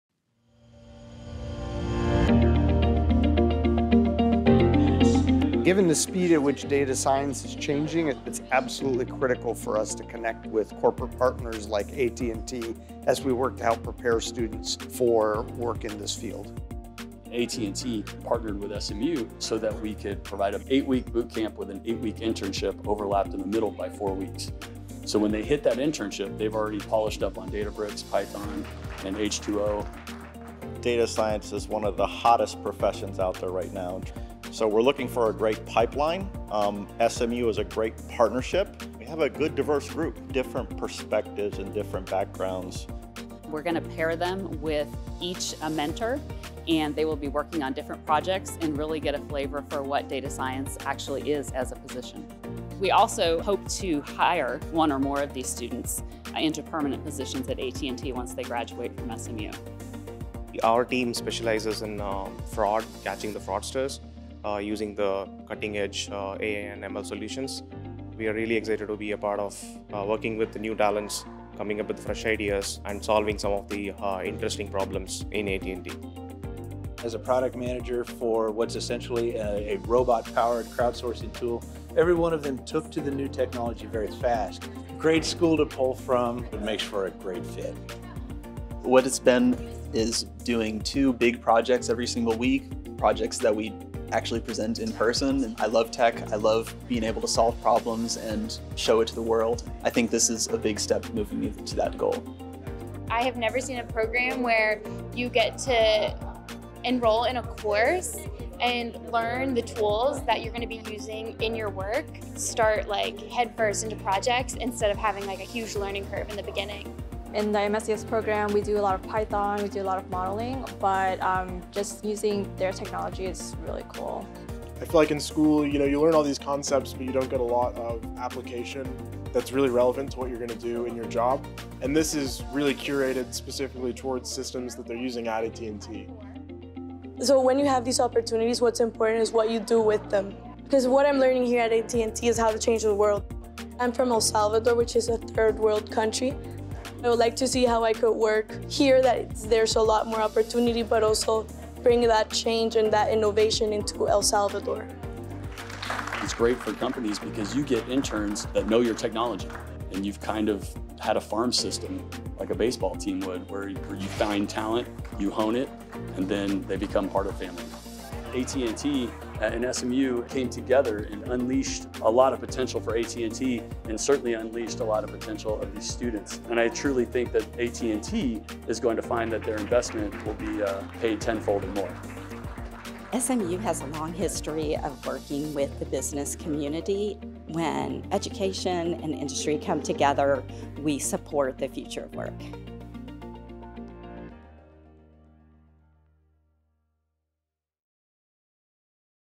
SMU Data Science Scholars Program
Bringing education and industry together, the SMU and AT&T Data Science Scholars program offers students an opportunity to use their data science or engineering talents to break down business problems by analyzing data and guiding improvements.

For more information about working with SMU to build your organization's employment pipeline, visit smu.edu/corporateprograms